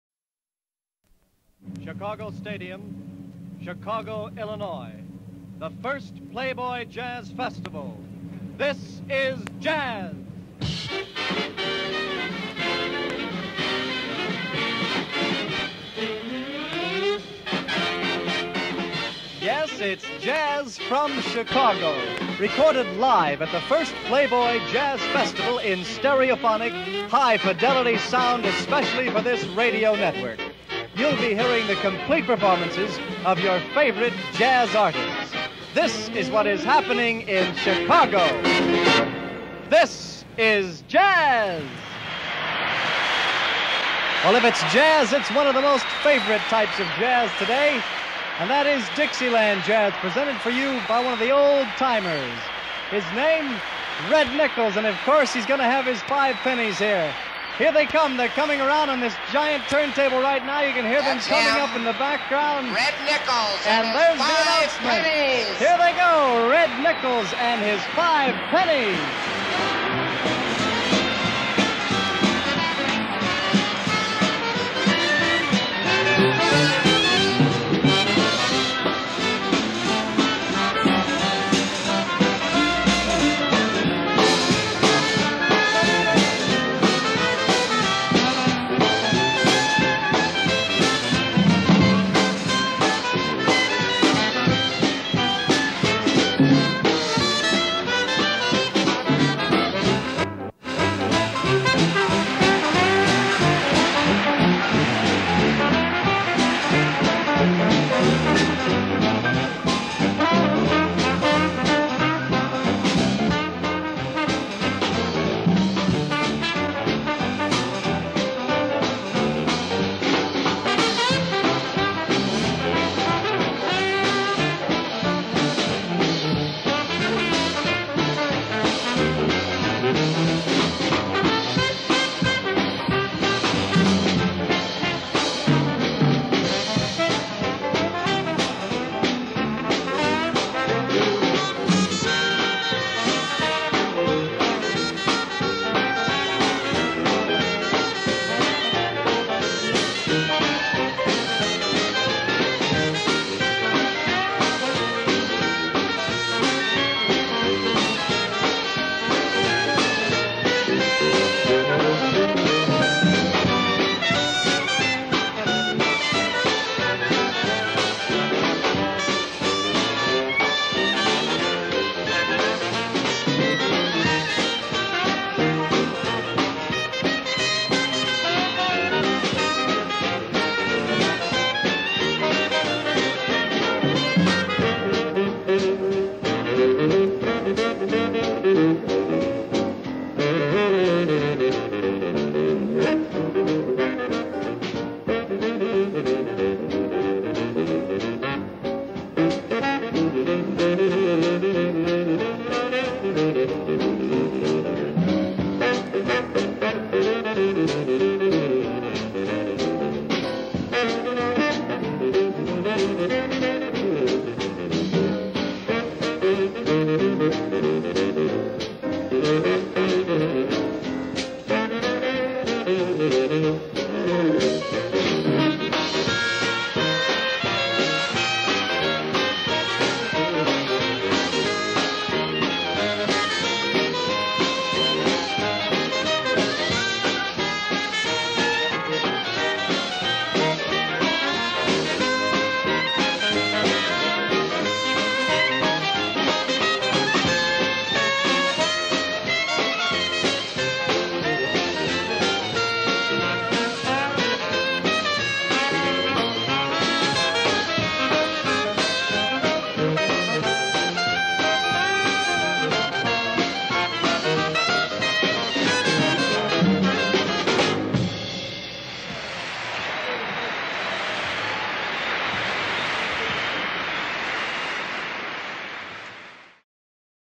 "Medley of 1920s Tunes" Red Nichols Five Pennies Playboy Jazz Festival Chicago 1959
Red Nichols Five Pennies from the first Playboy Jazz Festival from 1959 in Chicago.